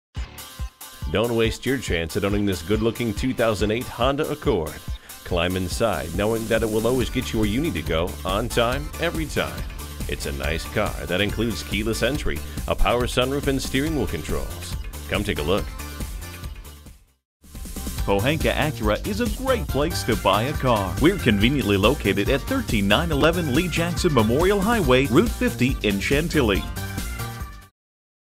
2008 Honda Accord Fairfax Acura Washington-DC, MD DC007959B - SOLD
Year: 2008
Make: Honda
Model: Accord
Trim: EX
Engine: 2.4L I4 DOHC i-VTEC 16V
Transmission: 5-Speed Automatic with Overdrive
Color: Green
Mileage: 66706
Stock #: DC007959B
Nice car! STOP! Read this! Don't waste your chance at owning this good-looking 2008 Honda Accord. Climb into this superb Honda Accord, knowing that it will always get you where you need to go, on time, every time. J.D. Power and Associates gave the 2008 Accord 4 out of 5 Power Circles for Overall Initial Quality Design. New Car Test Drive said, "...By model range, powertrain choices and overall efficiency, the Honda Accord line-up surpasses everything in its class. Measured by holistic performance or overall refinement, the Accord is difficult to beat..."

Address:
13911 Lee Jackson Hwy Chantilly VA